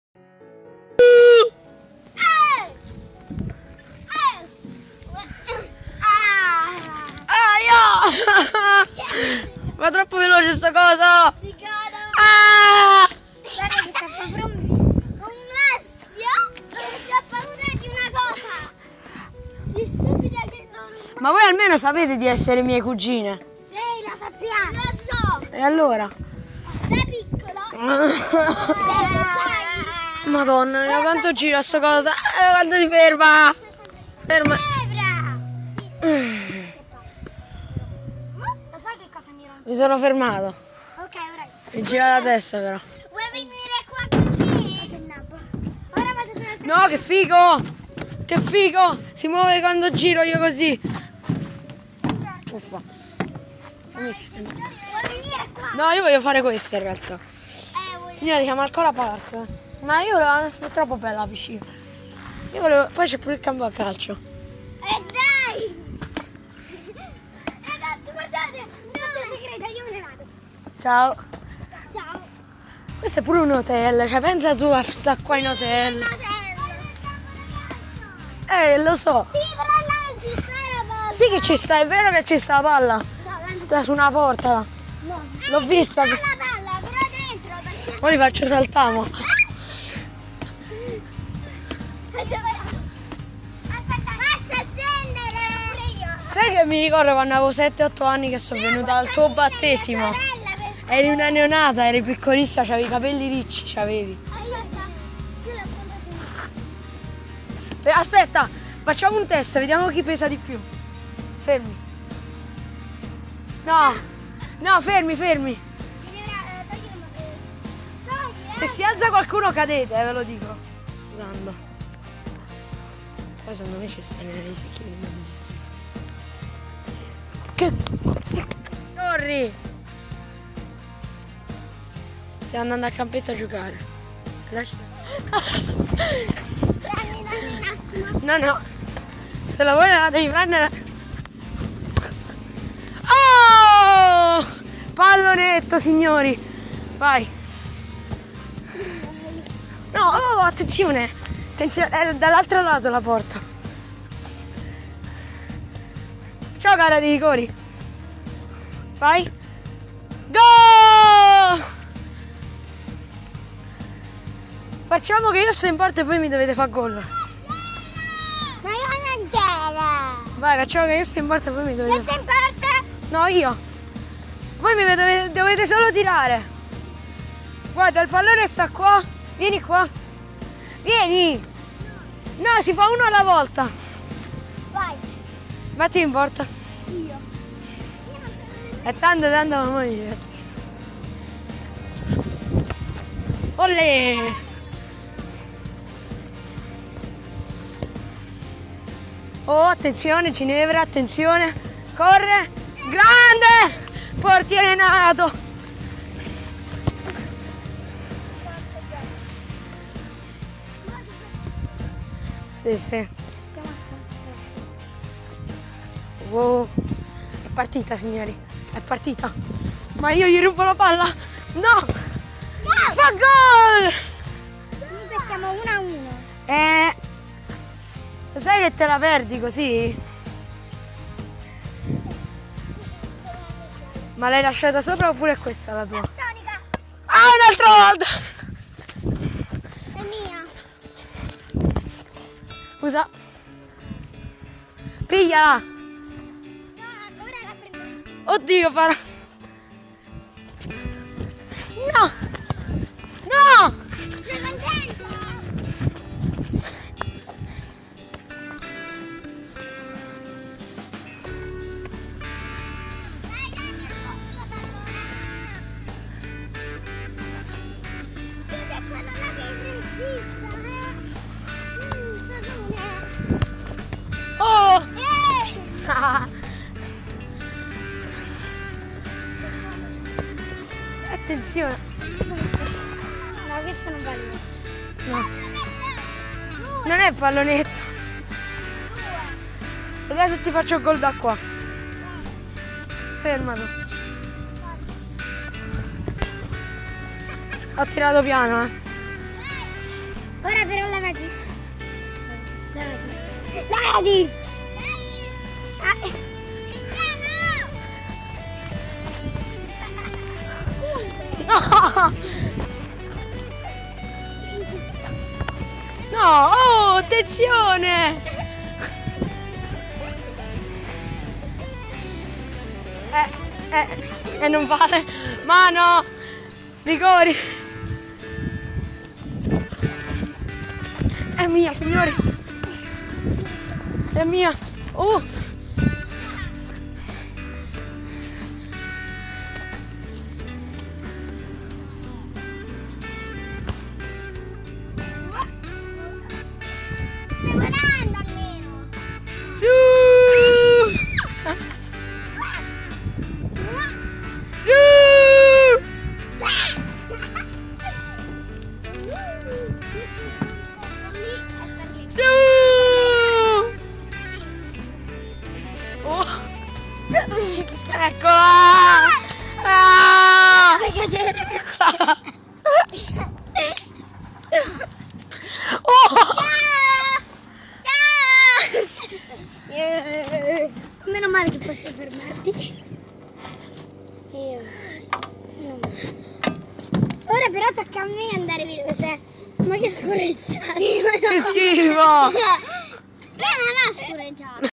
mai visto un posto così immenso
oggi siamo al kora park con le mie cuginette. siamo andati nell' area giochi, al capo da calcio, ma soprattutto abbiamo gustato una cucina stupenda del posto